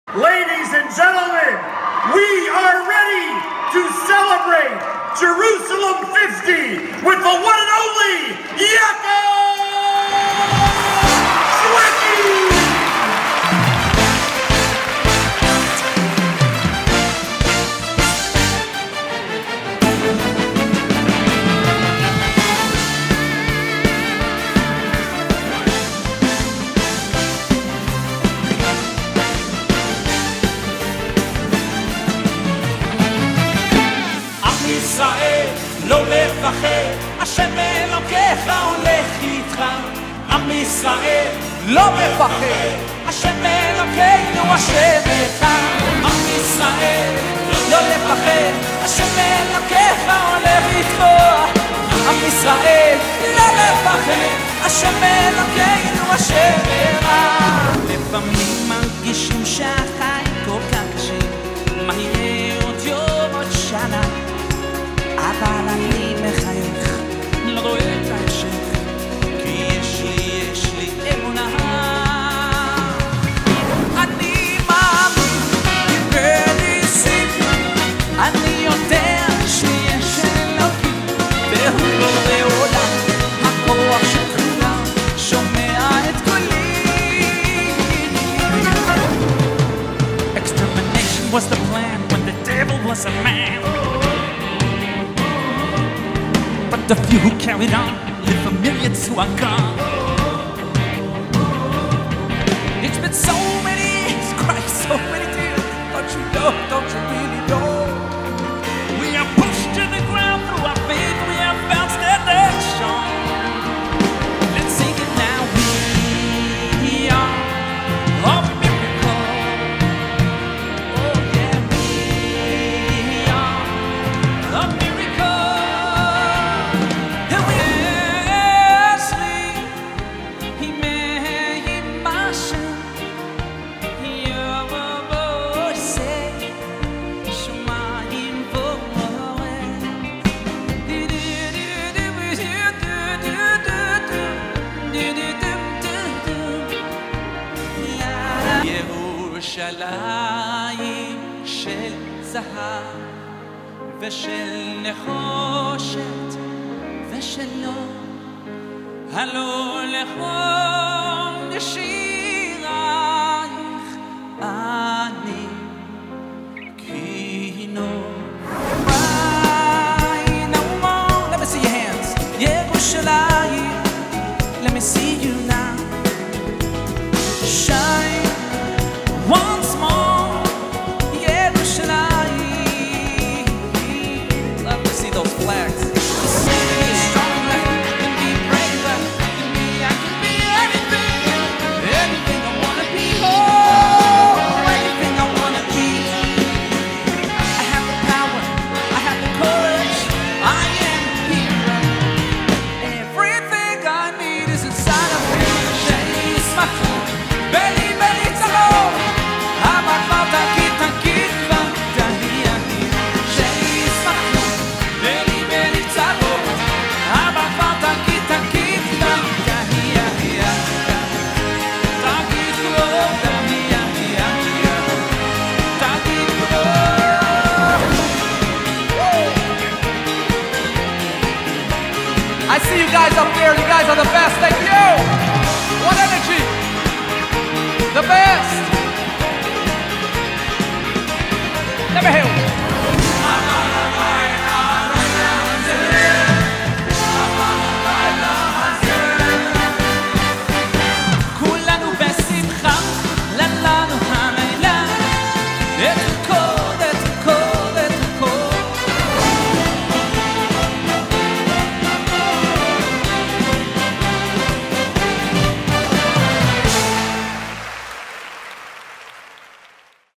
Yaakov Shwekey | Live in Jerusalem 2017 | Promo
Watch the recap of Yaakov Shwekey's latest show, performing at the World Mizrahi grand opening event at the ICC Jerusalem, commemorating the 50th year anniversary of the unification of Jerusalem, attended by thousands from both Israel and the Diaspora